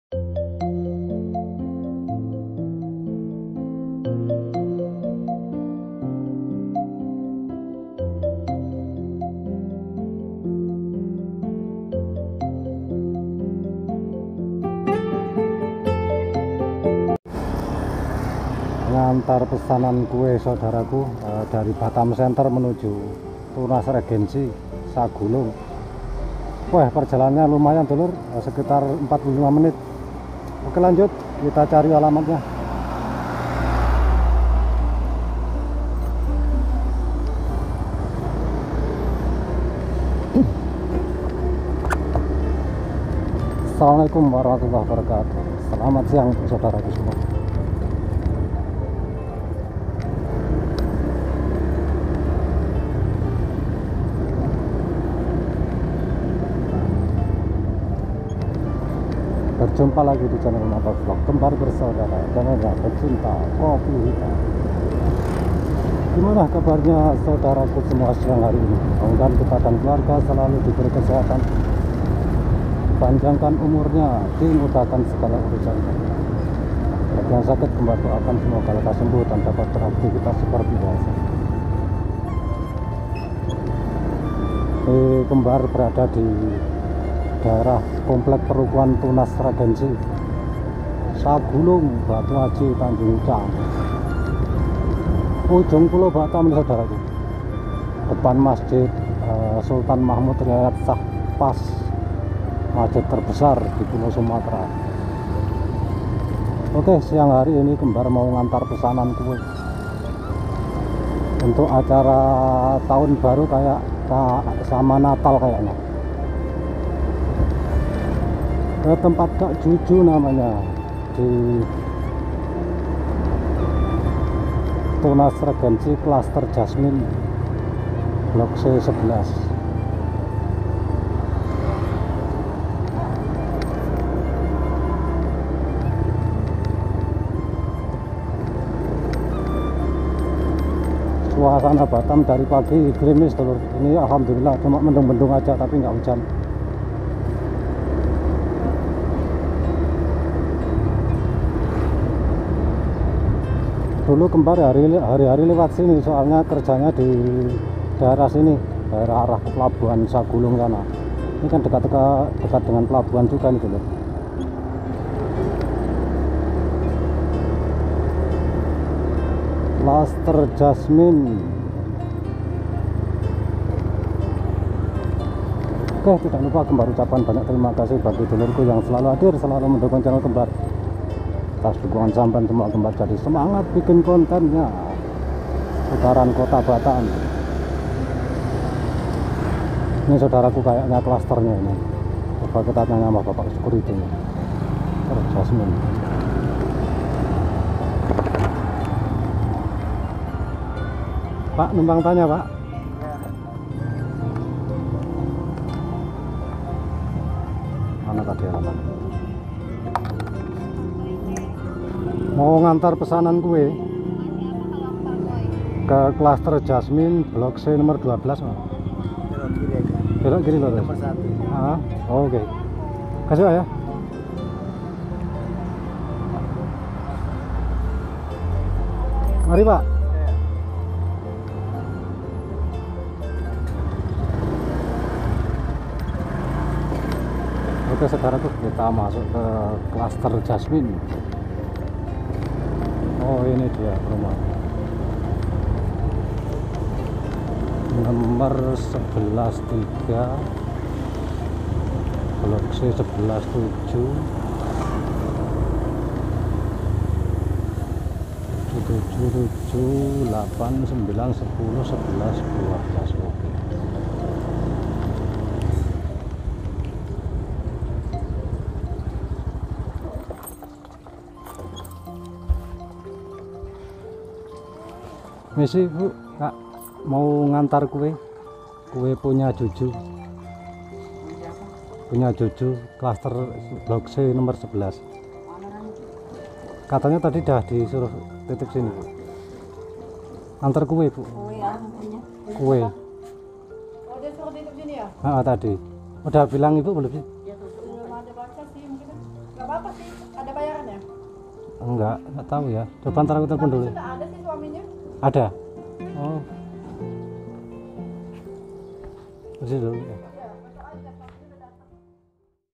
MOTOVLOG PEMULA !! NGURIR ANTAR KECAMATAN DALAM KOTA.30 km perjalanan 45 menit.tunas regency.
Selamat pagi siang sore dan malam untuk saudaraku semua.dari sabang sampai merauke semoga anda dan keluarga sehat dan bahagia selalu amin.
Siang hari yang indah cuaca mendung setelah di guyur hujan di daerah tunas regency,cluster jasmine tanjung ucang batu aji kota batam.
Dari batam centre menuju ujung pulau batam ngurir kue.pesanan untuk hari raya natal dan tahun baru.
Ruko2 tunas regency tanjung ucang yang indah dan megah.persis di depan masjid agung sultan mahmud riayad syah.masjid terbesar di pulau sumatera yang megah dan mewah.
Sebelum masuk cluster jasmine melewati perumahan dan konplek2 ruko.
Selamat menonton.
Semoga terhibur.
Dari awal hingga akhir banyak kesalahan mohon di maafkan.
Cukup sekian dulu vidio dari kembar bersaudara.
Lain hari lain waktu kembar sambung lagi.
Terima kasih .
Wassallamualaikum wr wb.